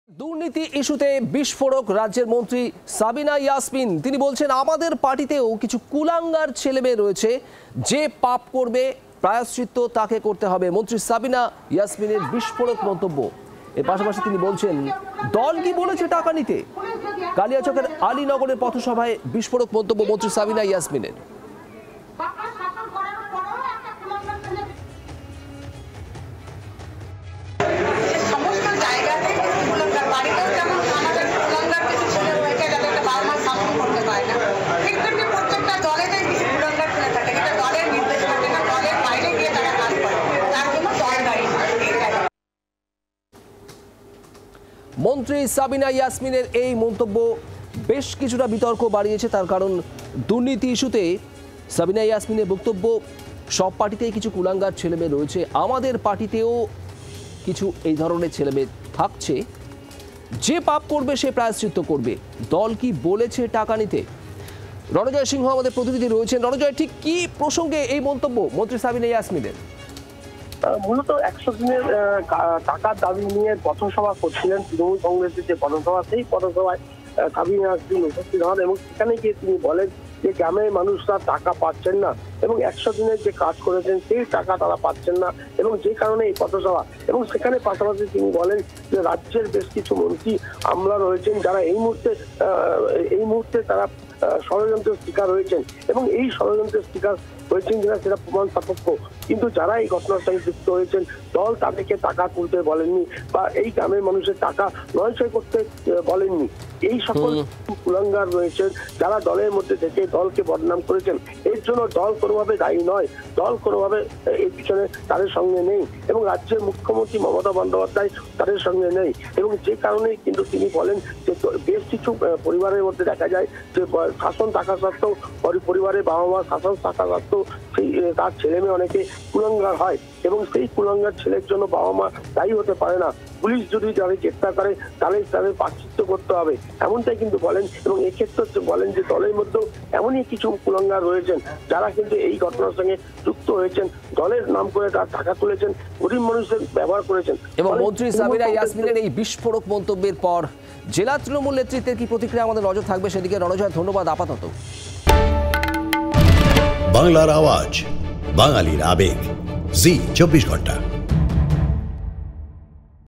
Sabina Yasmin: 'দল কি বলেছে টাকা নিতে?' বিস্ফোরক মন্ত্রী সাবিনা ইয়াসমিন! | Zee 24 Ghanta
Sabina Yasmin: 'দল কি বলেছে টাকা নিতে?' বিস্ফোরক মন্ত্রী সাবিনা ইয়াসমিন! দেখুন তিনি কী বলেছেন... | Zee 24 Ghanta